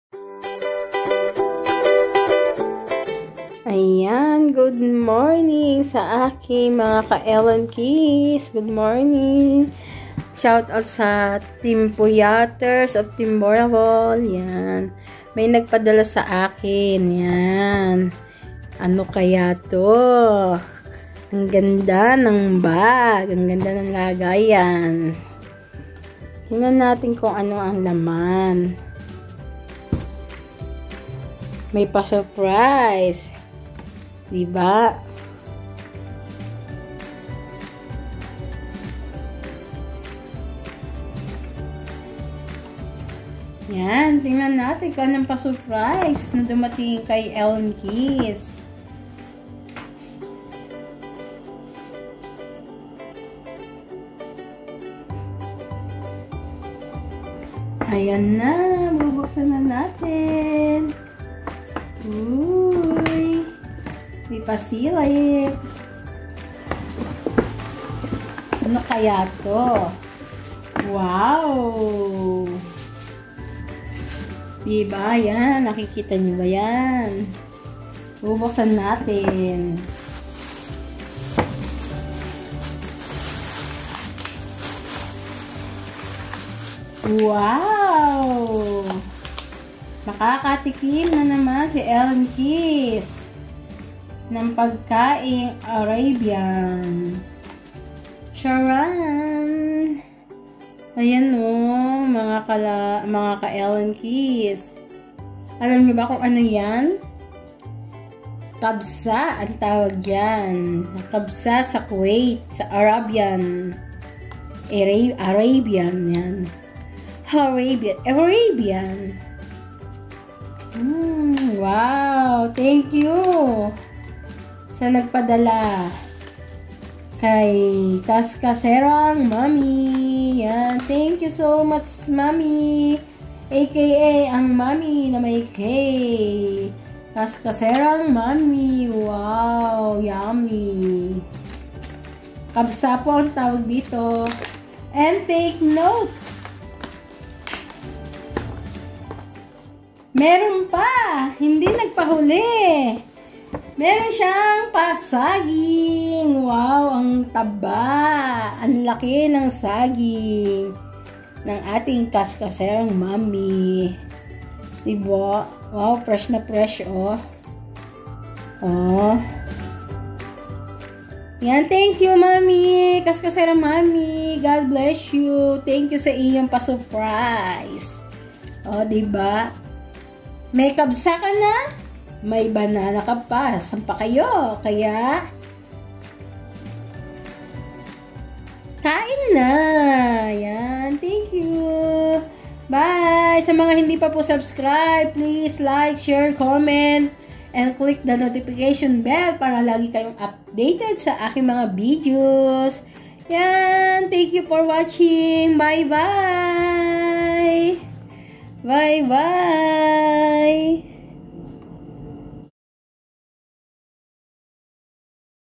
Morning Unboxing Kabsa Arabian Food with Banana from K.Mami. Kain Po Tayo.
thank you sa Napakasarap na Kabsa na regalo mo saken with Special deliver pa @kaskaserangmami .